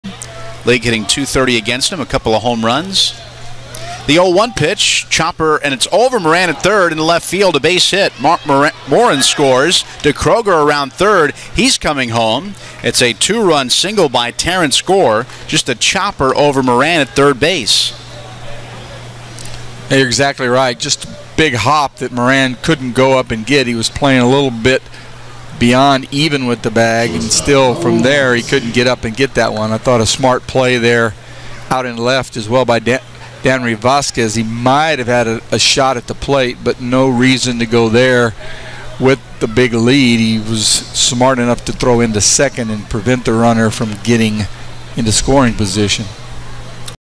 Gore drives two in for Naturals
8/23/15: Northwest Arkansas' Terrance Gore slaps a two-run single to left as the Naturals fall, 6-2, to Corpus Christi